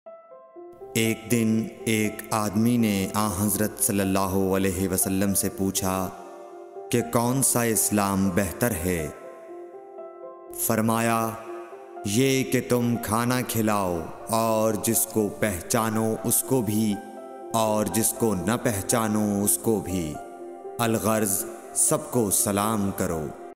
SAHIH BUKHARI HADEES 12 URDU TRANSLATION
In this video, we delve into Sahih Bukhari Hadees 12 and provide a detailed explanation of its meaning and significance. Sahih Bukhari is one of the most authentic collections of sayings and actions of Prophet Muhammad (peace be upon him), making it a valuable source of knowledge for Muslims around the world. Join us as we explore the wisdom contained in Hadees 12 and uncover its teachings for our daily lives. Don't miss out on this enlightening discussion!